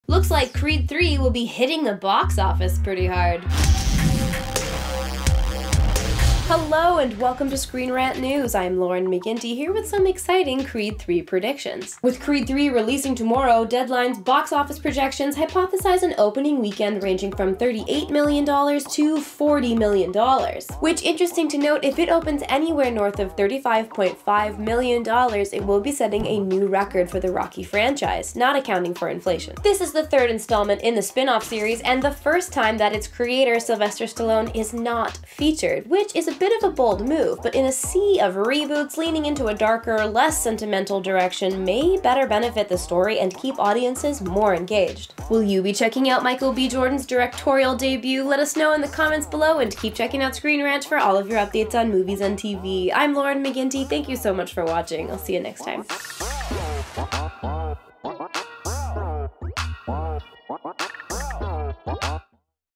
Creed 3 Box Office Headed For A Rocky Franchise Best Opening Weekend!
Creed 3's box office projections have Michael B. Jordan's movie headed for the biggest opening weekend in the history of the Rocky franchise.